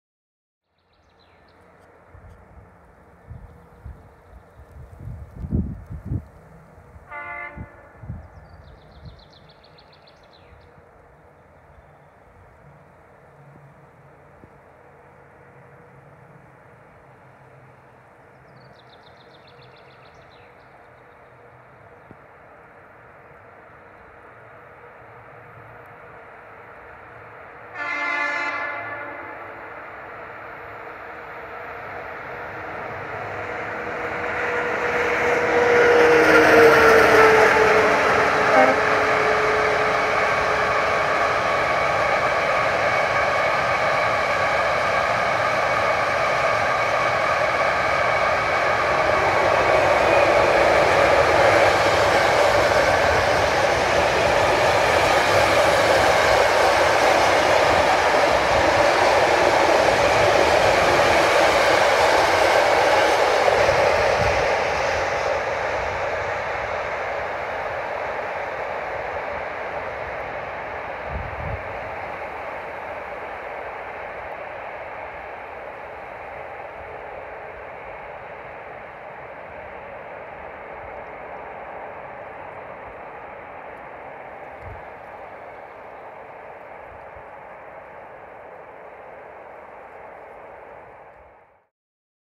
[ČD Cargo Poland] 163 041-7 | Boleń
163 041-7 ze składem platform i wagonów Tams jako 484026 Chałupki - Szczecin Port Centralny SPB12 złapana na szlaku Zbąszynek - Szczaniec. Podziękowania dla mecha za sygnały „baczność” tym samym za 300 subskrypcji na moim kanale :)

Linia kolejowa: 3 Warszawa - Kunowice